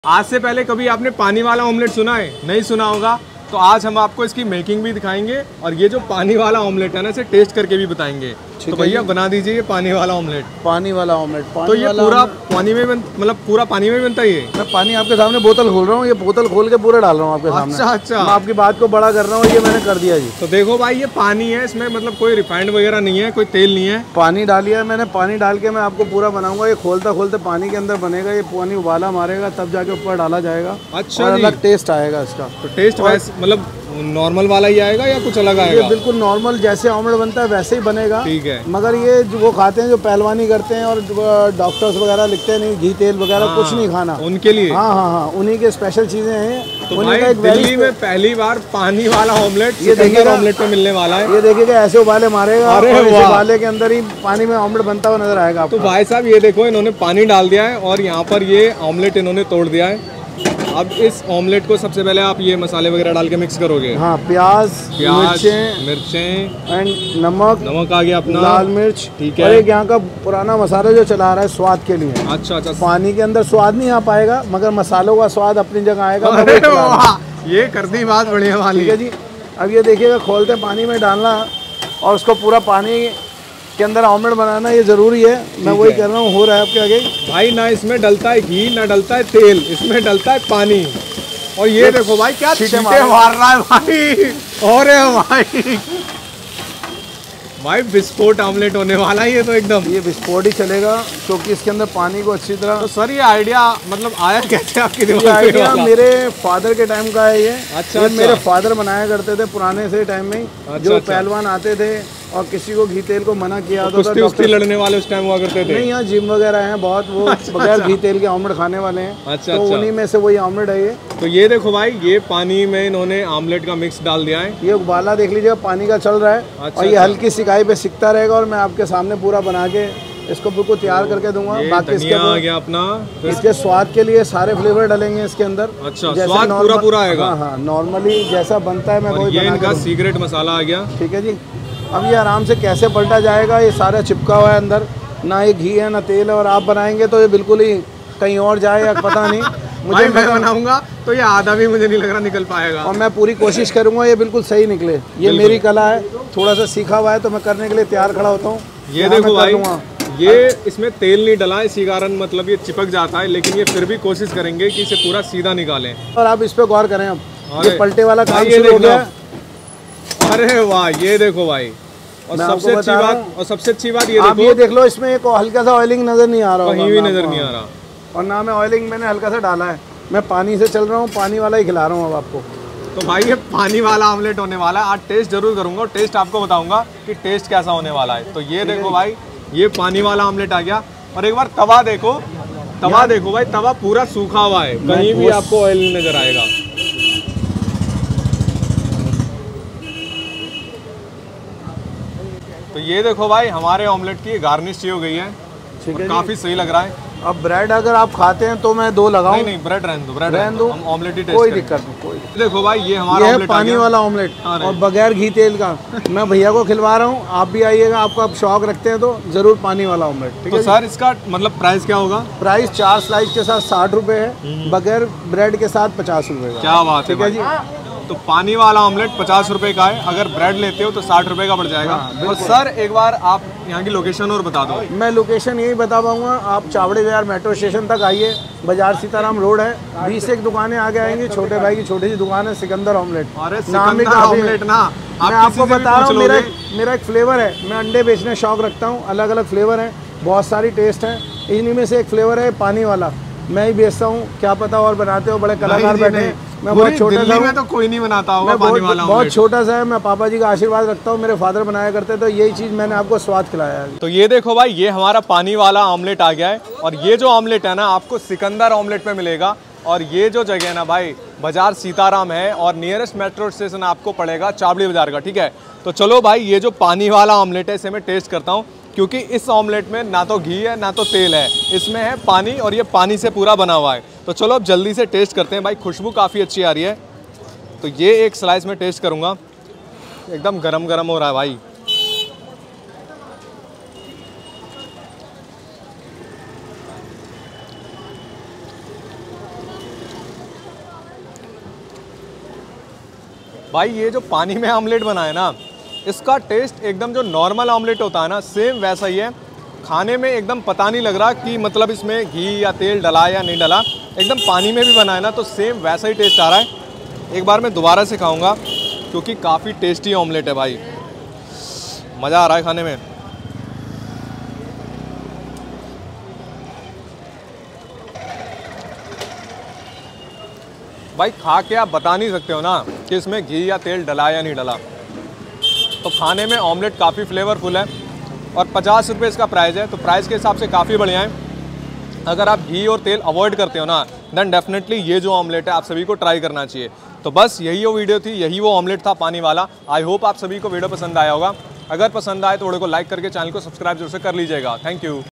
भारत का पहला पानी वाला ऑमलेट😱😱 || ना डलता है तेल ना डलता है घी ये Omlate बनता है पानी से
kya kabhi apne paani wala omlate khya hai agar nahi tho aaj me apko dikhaunga ki ye paani wala omlate kaisa hota hai

address : Sikandar Omlate Wala, Bazaar Sita Ram, Nearest Metro Station Chawari Bazar